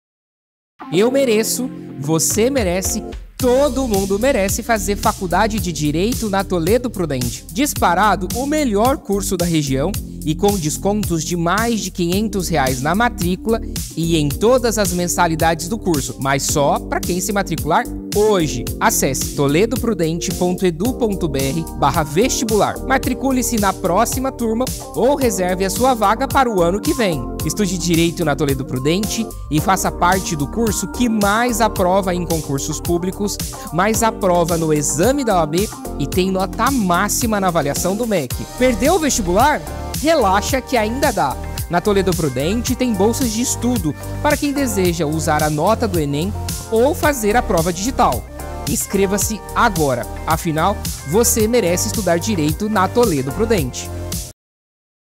Faça Direito na Toledo Prudente ainda em 2023!
Todo mundo merece fazer Direito no melhor curso da região, ou seja, na Toledo Prudente.

Mas você só tem até HOJE para se inscrever no Vestibular da Toledo Prudente. Acesse agora o nosso site e saiba mais!